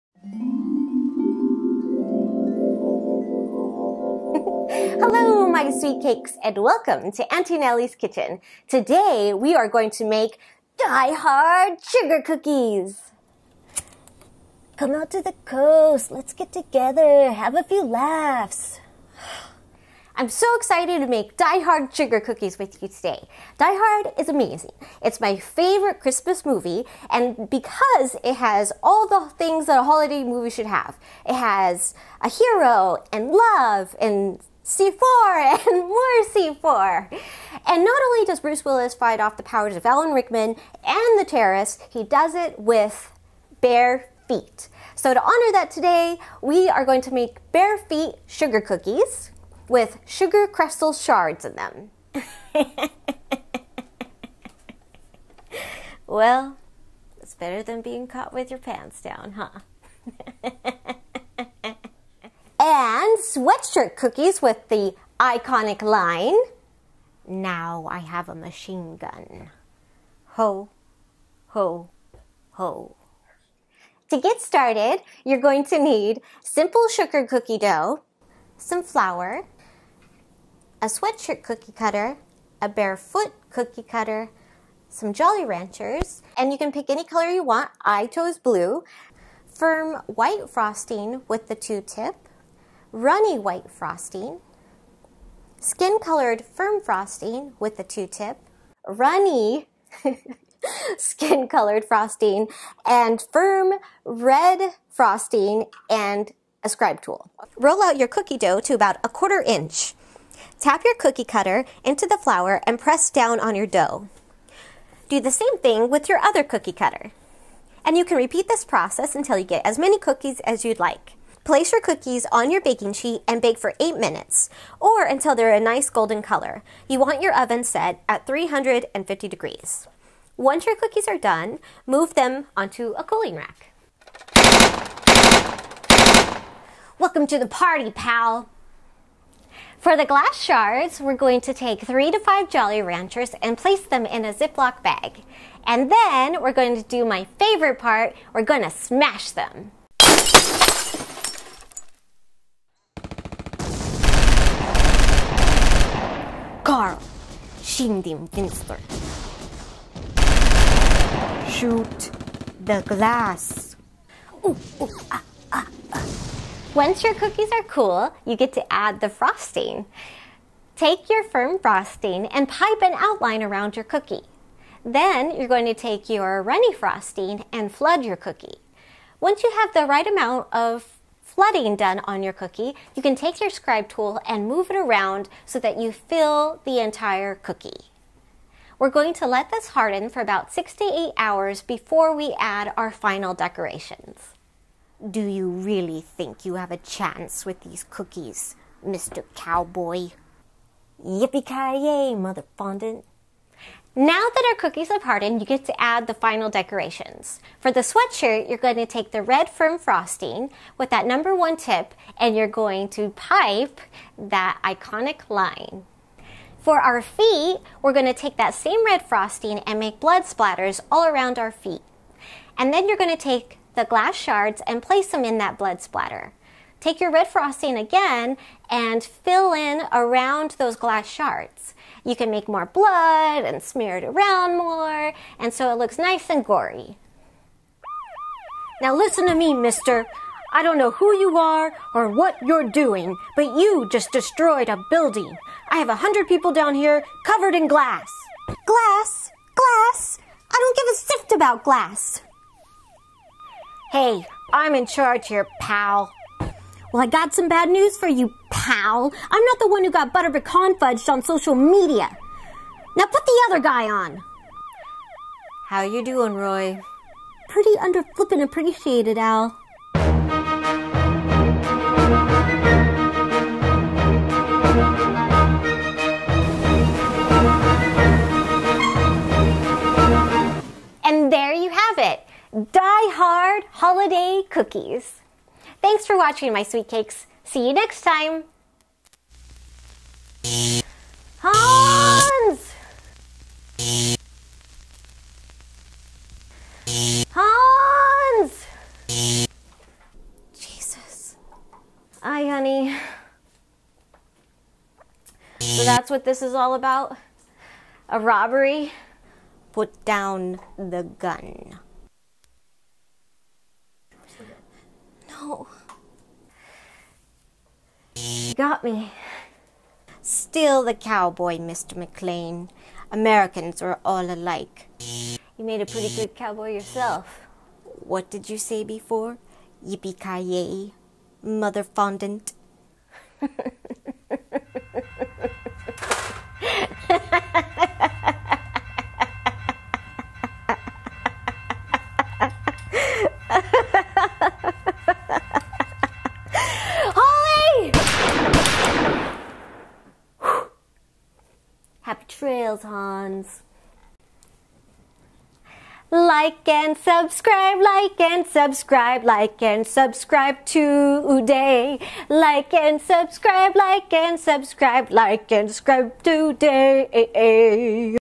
How to make Die Hard Sugar Cookies
Is Die Hard a Christmas movie?  Yes.  Is it the best Christmas movie?  Also yes and you know these cookies are delicious.

Ingredient

Simple Sugar Cookie Dough
Flour
Sweater Cookie Cutter
Foot Cookie Cutter
Baking Sheet
Spatula
Blue Jolly Ranchers
Firm White Royal Frosting
Runny White Royal Frosting
Firm Skin or close to Skin Colored Royal Frosting
Runny Skin Royal Frosting
Firm Red Frosting
Piping Bags
Piping Tips 2 & 3

Flour your surface. Roll out your cookie dough
to about a 1/4 in. Tap your cookie cutter into
flour and press down on your dough. Do the
same thing with your other cookie cutter.
Repeat this process until you get as many cookies
as you’d like. I like to make extras just for me.
Place your cookies on your baking sheet and bake
for 8 minutes or they are a nice golden color in
an oven set at 350 Degrees. Once your cookies
are done move them onto a cooling rack.

We want to make glass shards. Place 3 to 5 blue
jolly ranchers in a ziplock bag and lay them down on
a sturdy surface, and my favorite part smash them
with a hammer!

Once your cookies are cool you can add your frosting.
You want two kinds of Royal Frosting “Firm” for piping
and “Runny” for flooding the cookie. We need to pipe
an outline of the cookie and then fill with the Runny Frosting.
And repeat this on the other cookies. And let the frosting harden.
Now that our cookies have hardened we can
add the last of our decorations. For the sweatshirt
take your Red frosting and pipe:
Now I have a machine gun. Ho. Ho. Ho.
Our feet we want to take our firm red frosting and
pipe splatter marks all around and then sprinkle
those marks with our broken jolly ranchers.